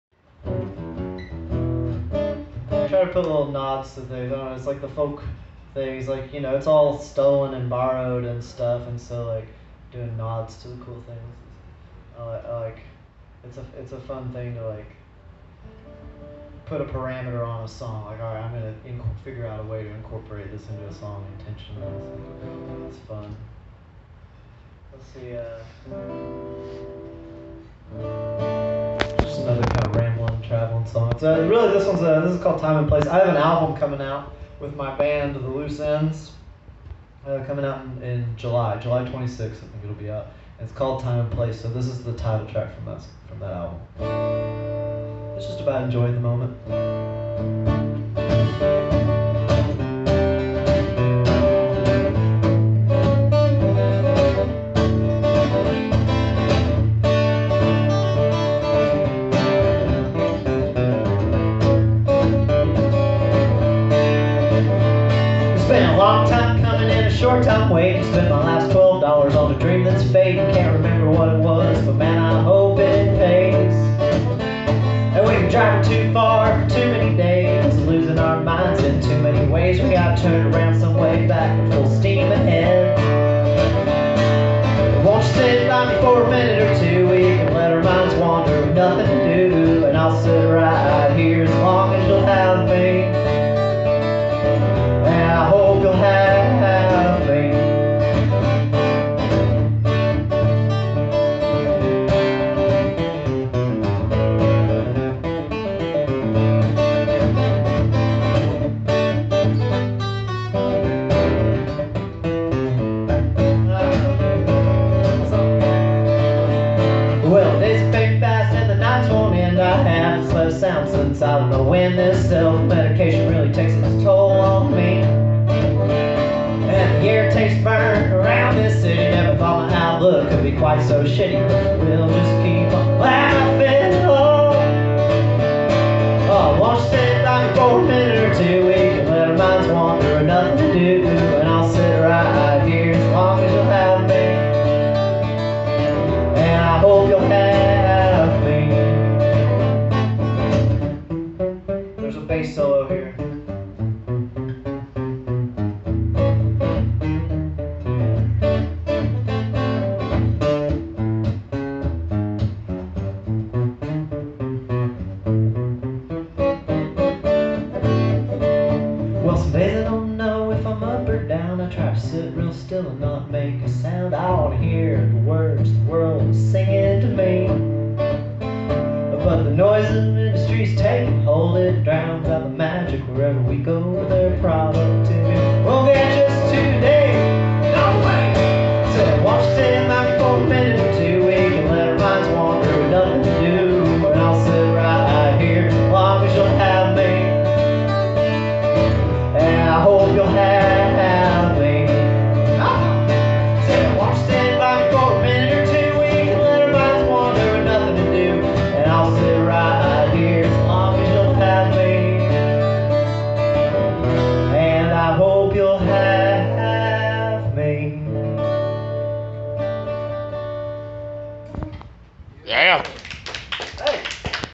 6-14-24 Jacob Price Zazz - Live at NCom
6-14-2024. Jacob Price Zazz performing live at NCom Cyberhall in Norman, OK.